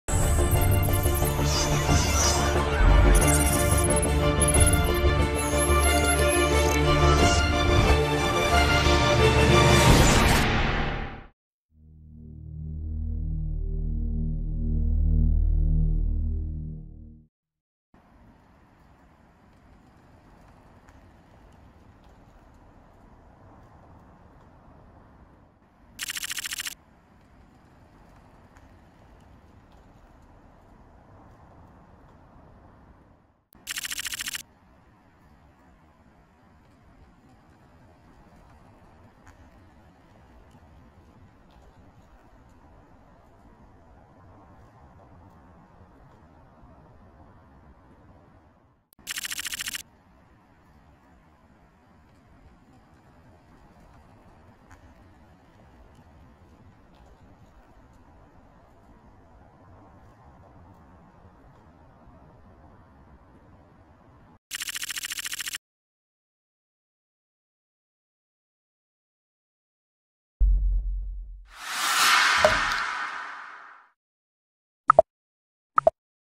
UFO SIGHTING FRANCE OCT 2021
The footage for this sighting is very short but i thought it was worth a look